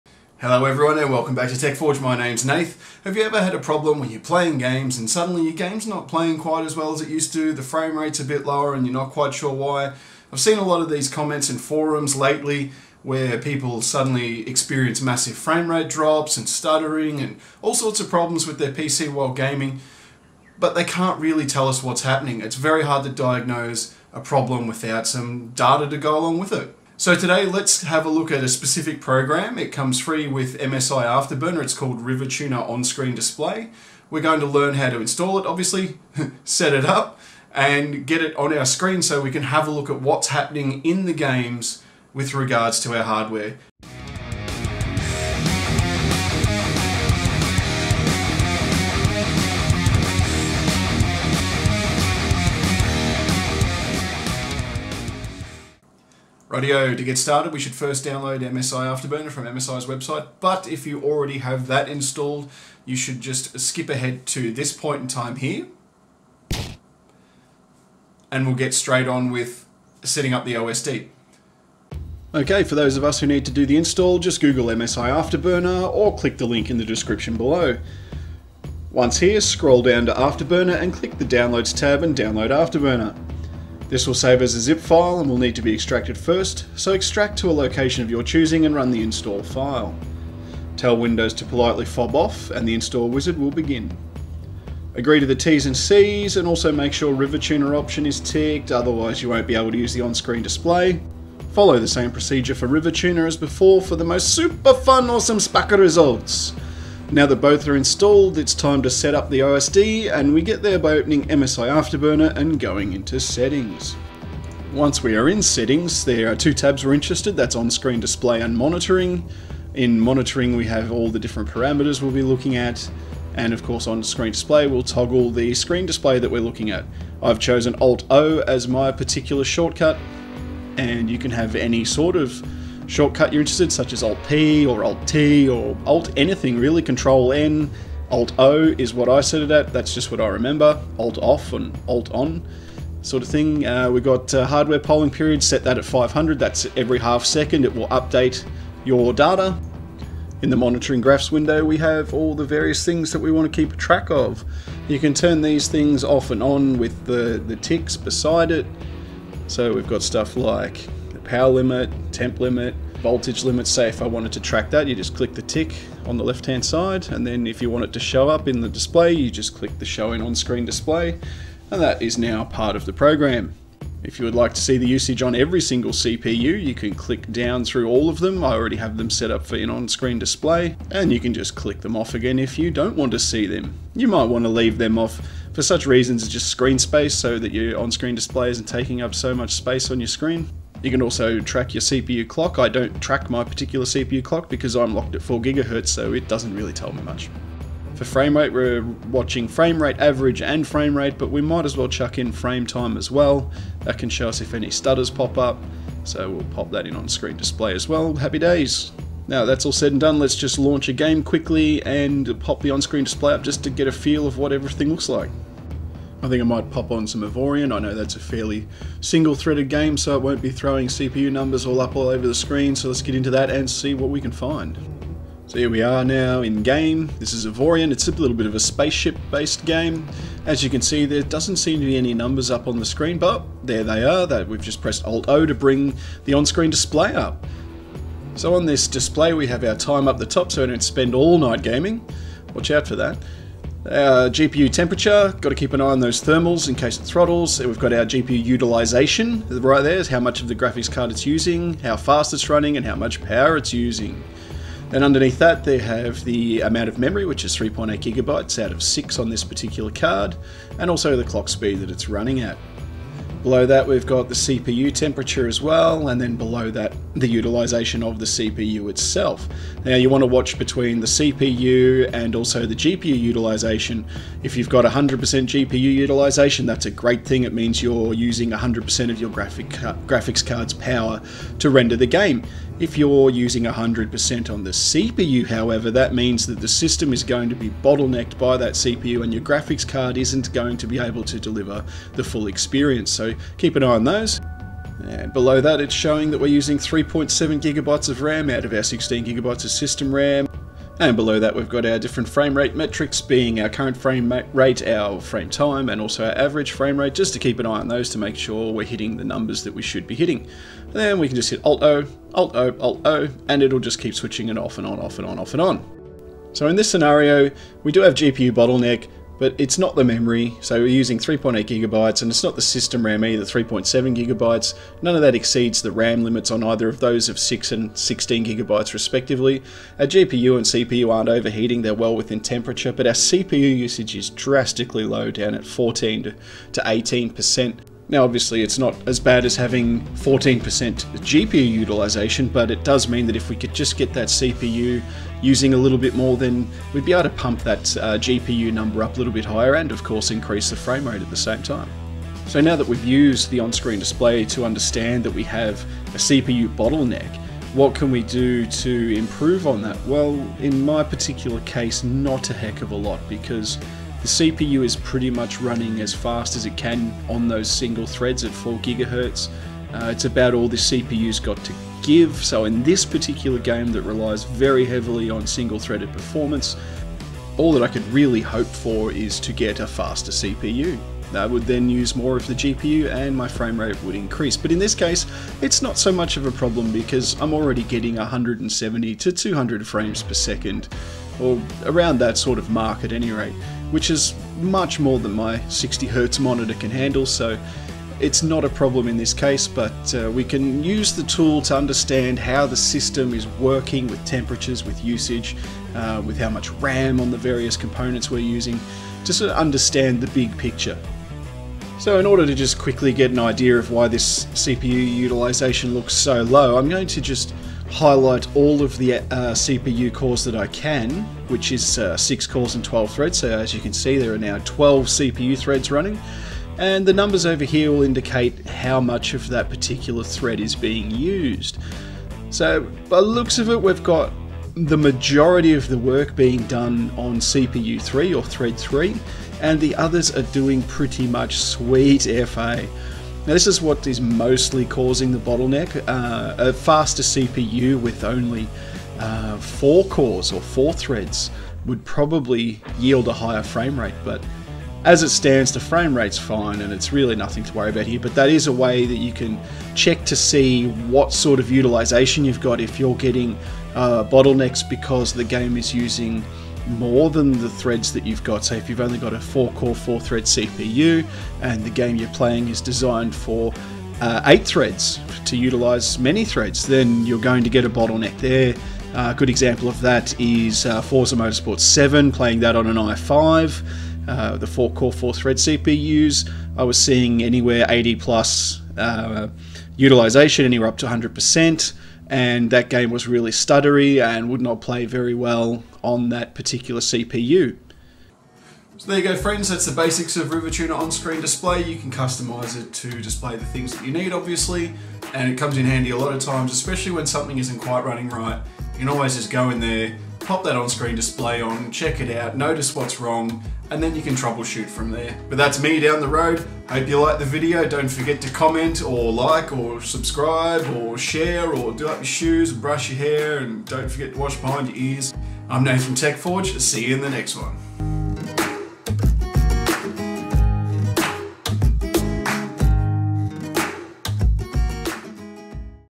HOWTO setup and read RivaTuner On Screen Display (OSD)
If you have ever wondered what your hardware is doing while gaming and wanted to know how to find out- play this video. Performance monitoring can help us identify bottlenecks and troubleshoot hardware issues, so every gamer should have their OSD setup to give vital system metrics.